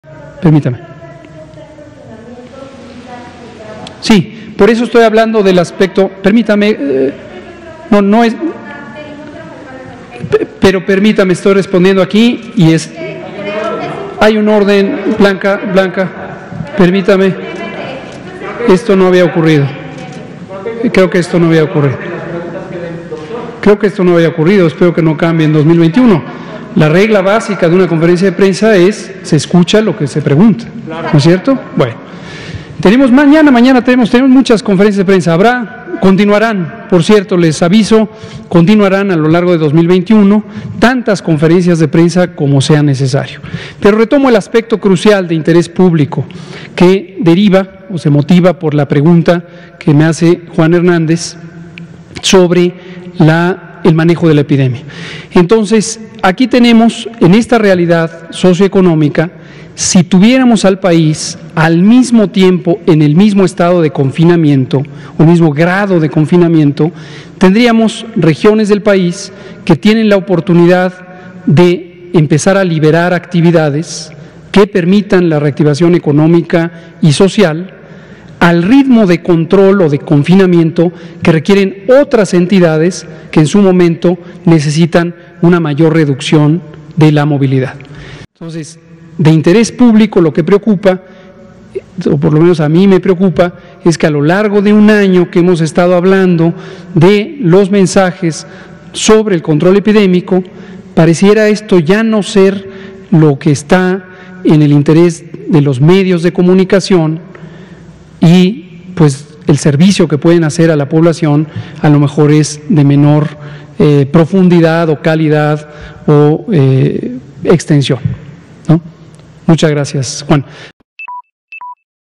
El DrMuerte llega tarde y se enoja si lo interrumpen y después regaña a los medios
El DrMuerte llega tarde y se enoja si lo interrumpen y después regaña a los medios por no dar un servicio de "profundidad o calidad” de comunicación sobre la pandemia 🤦🏻‍♀️